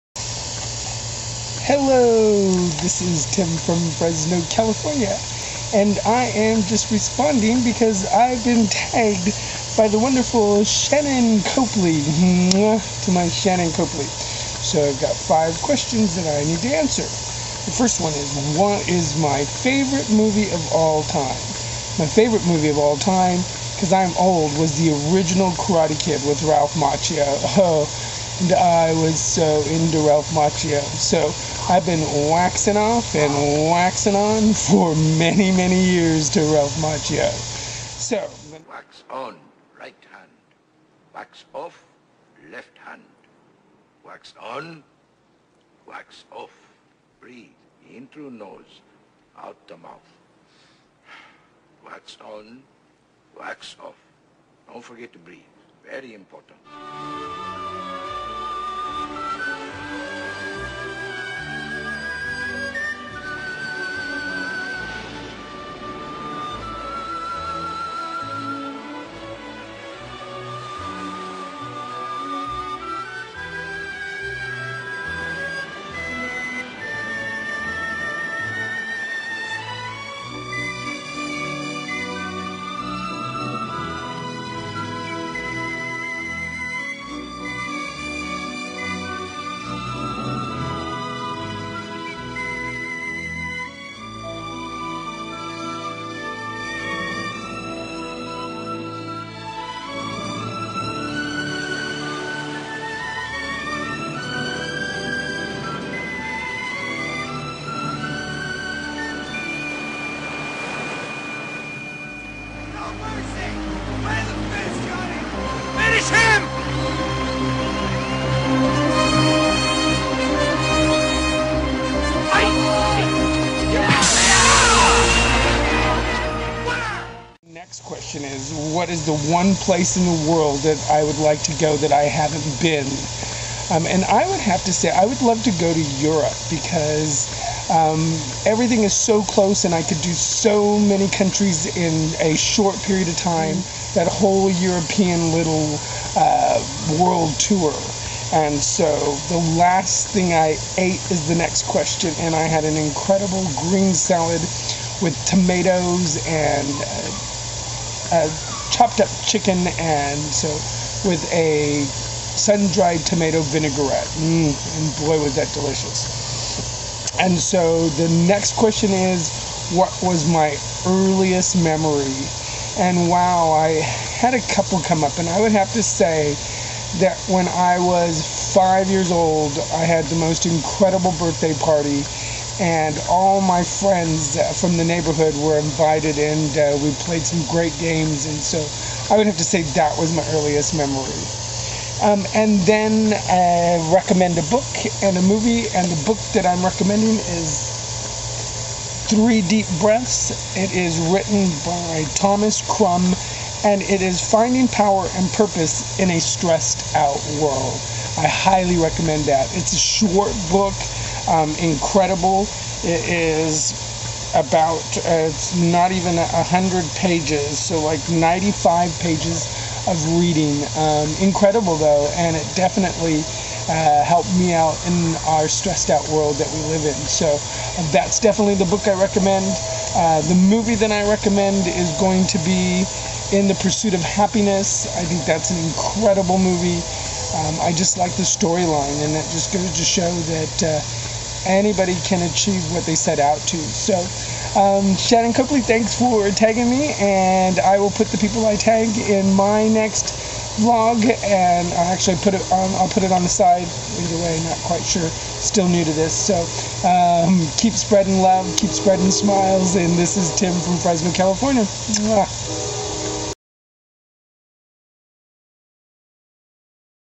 RE: 5 Questions Tag
I have been tagged 4 the 1st time! Thanks shannoncopley...this was fun! Hope u (and everyone else who sees this response)enjoys my responses!

I am not sure if u guys wanna play TAG but TAG its ur turn
justjoshfunk1
sranxelli
LaiYouttitham
gersing
IdeaParalytic